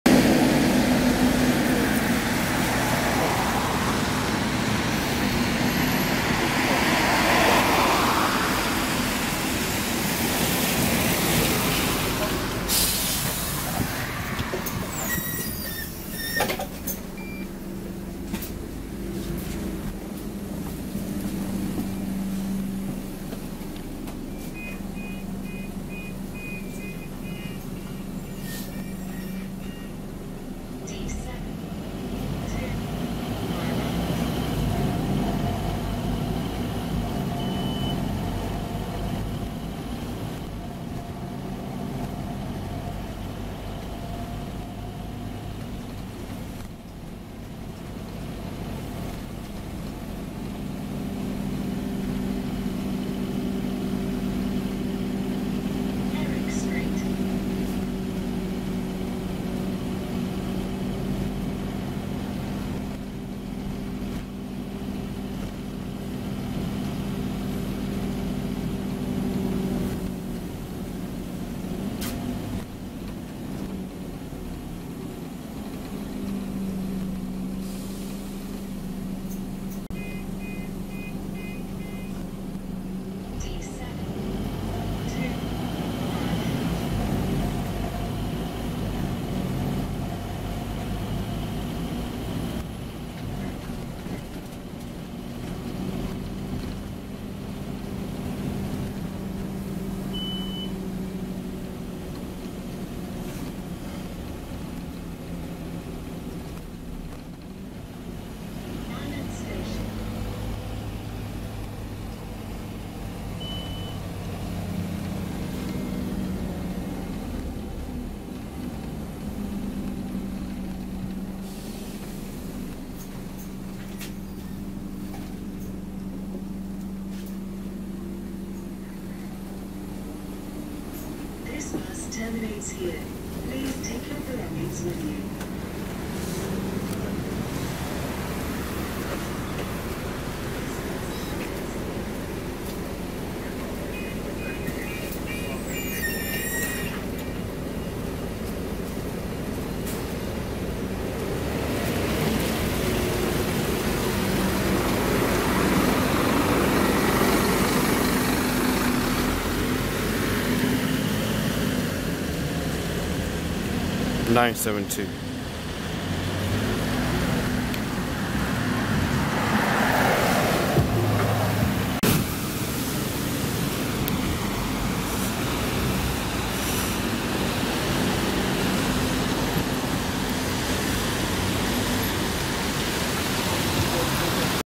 Scania Omnicity N230UD Go Ahead London Docklands Buses 972 YT59DYV on the Route D7 *Short Ride*
Route D7 from Ackroyd Drive to Mile End Station.